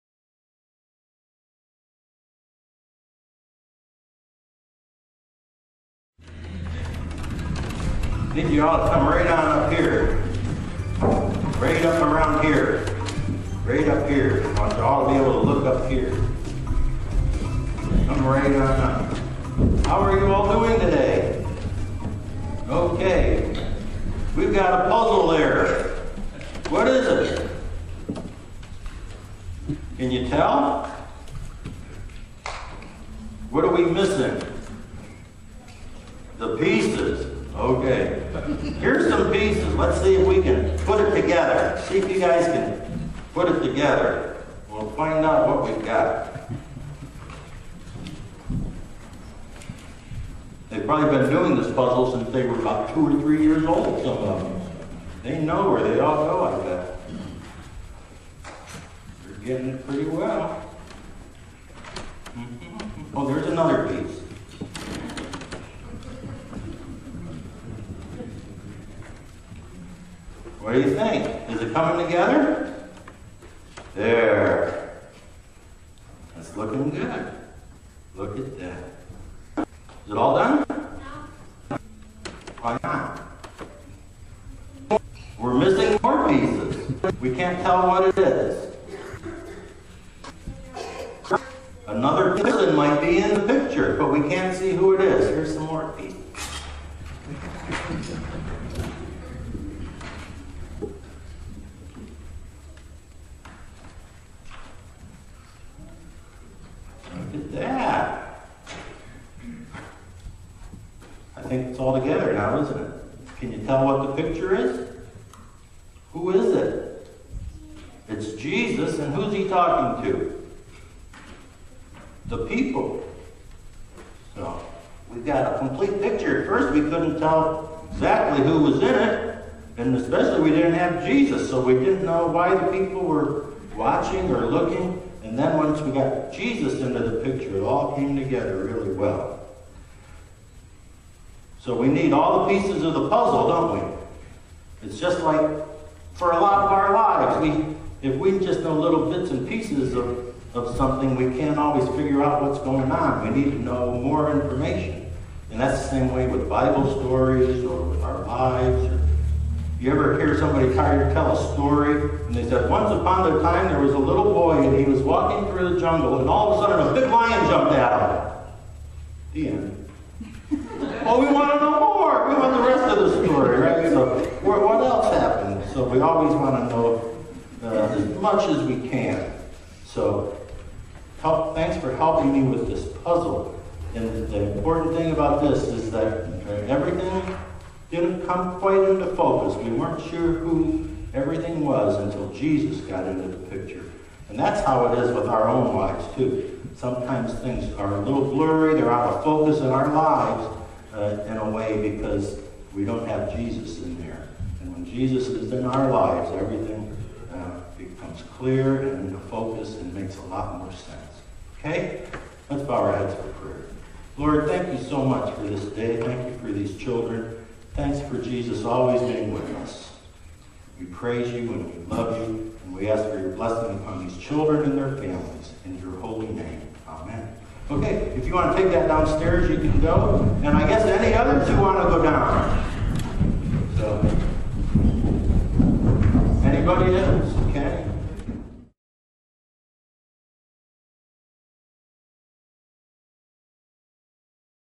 Paulding UMC April 18: Childrens Chat.wmv
Until Jesus is in the picture, sometimes the puzzle just doesn't make sense!  So it is with our lives, too.